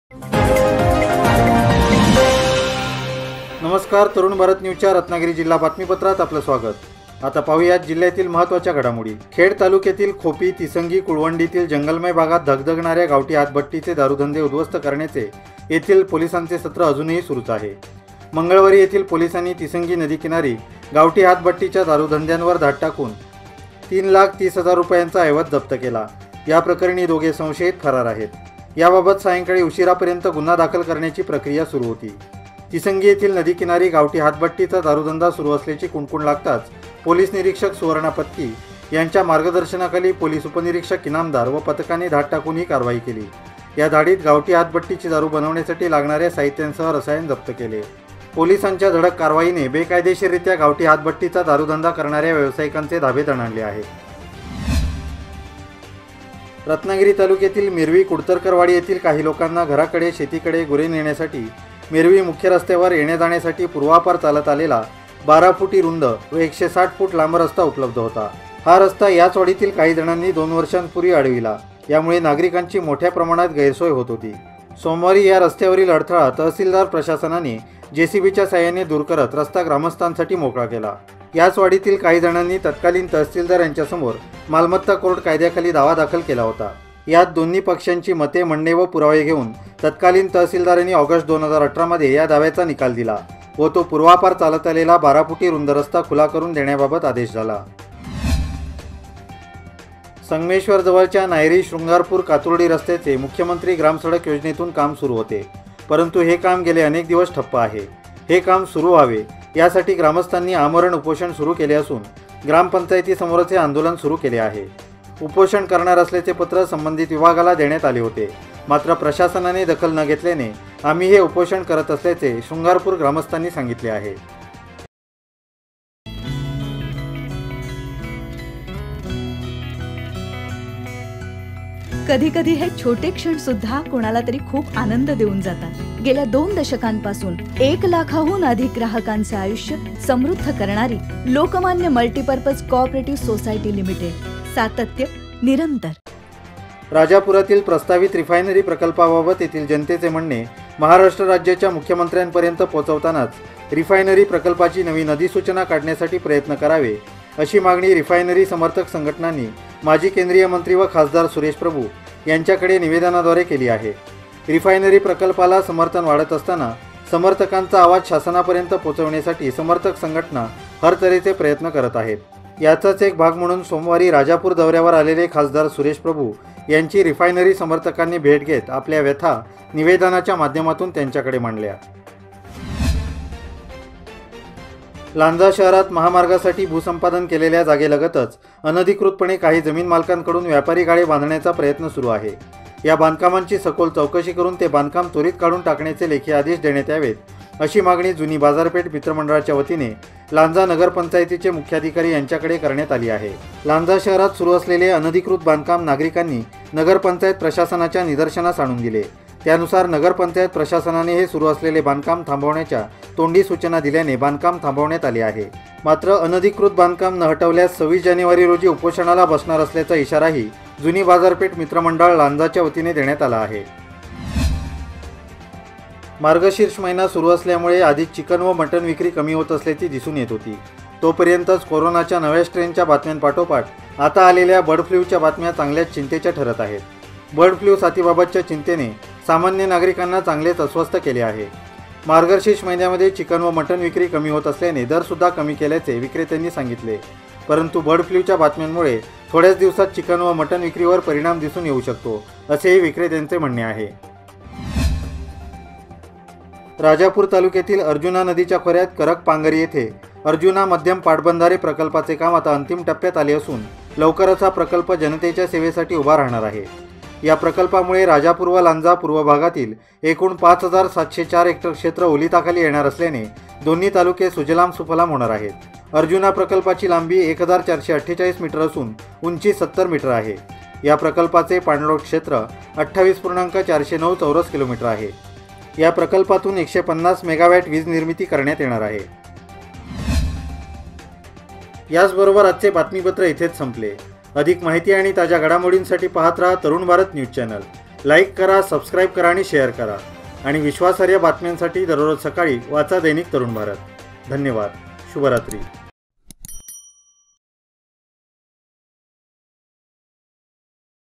रत्नागिरी न्यूज बुलेटिन | Ratnagiri News Bulletin | 05-01-2021
तिसंगीत दारूधंद्यावरील धाडीत 3 लाखाचा ऐवज जप्त
मेर्वी कुडतरकरवाडीतील रस्ता झाला अडथळा मुक्त
नायरी ते शृंगारपूर कातुर्डी रस्ता करण्यासाठी नागाfरकांचे उपोषण
राजापुरातील रिफायनरी प्रकuपाची नवीन अधिसूचना काढण्यासाठी प्रयत्न करा
लांजातील अनधिकृत बांधकाम हटवण्याची मागणी
बर्ड फ्लtच्या बातम्यांमुळे चिकन, मटण विक्रीला फटका
अर्जुना मध्यम प्रकuपाचे काम अंतिम टप्प्यात